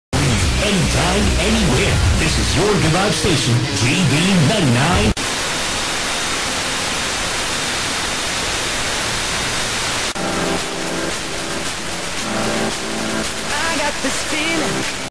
GVFM 99.1 Pampanga & 99.9 Batangas
Antenna used: Headset
Signal Location: Cabuyao, Laguna

List of Stations:
DWGV-99.1 FM-GVFM-Angeles
DZGV-99.9 FM-GVFM-Batangas*

*Co-interference with Radio Chinoy Manila